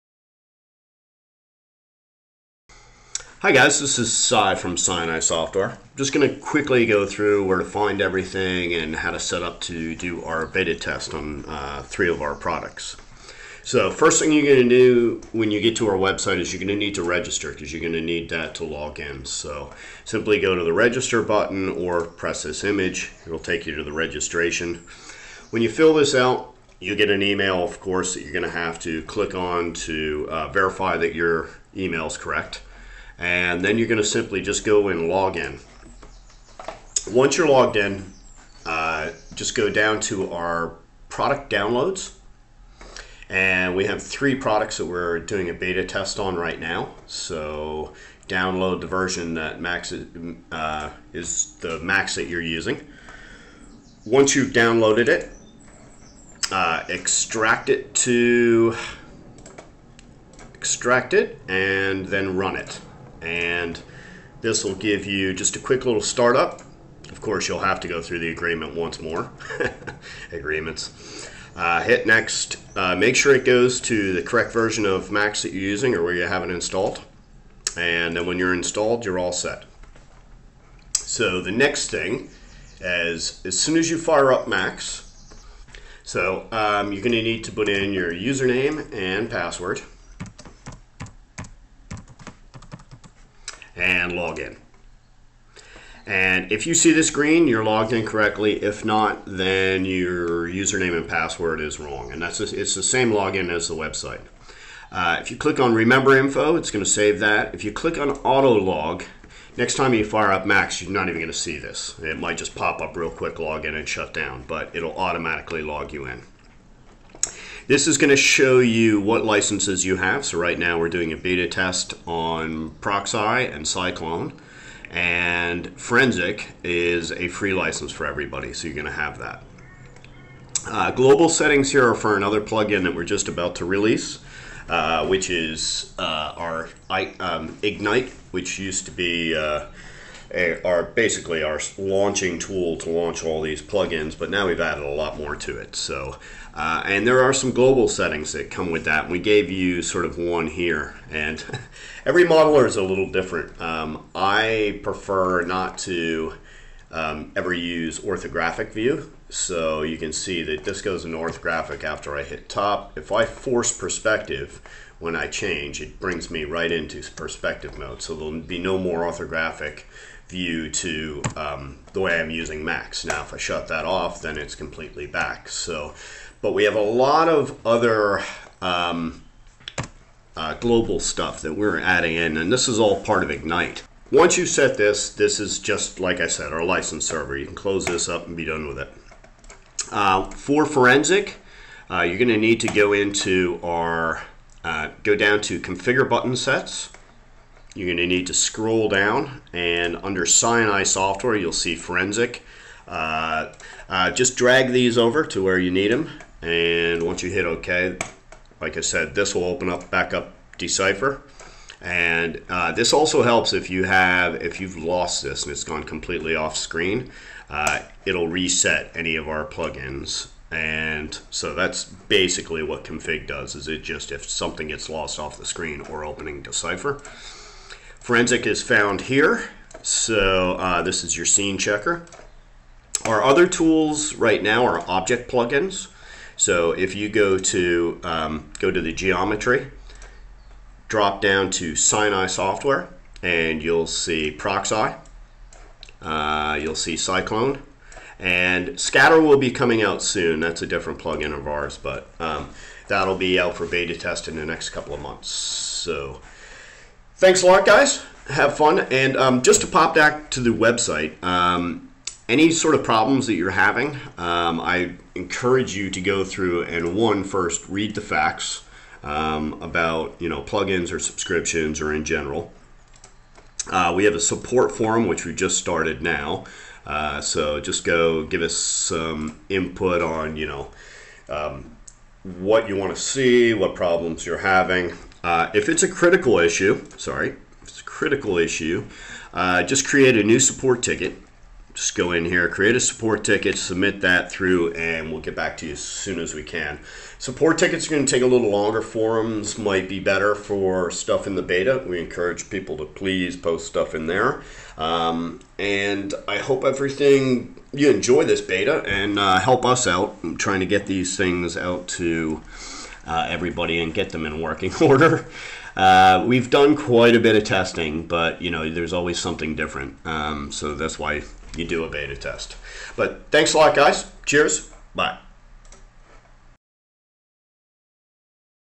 SiNi Software - Getting Started
Software Version - Autodesk 3DS Max

The video demonstrates the procedure to register, login and start downloading our plugin products for Autodesk 3DS Max.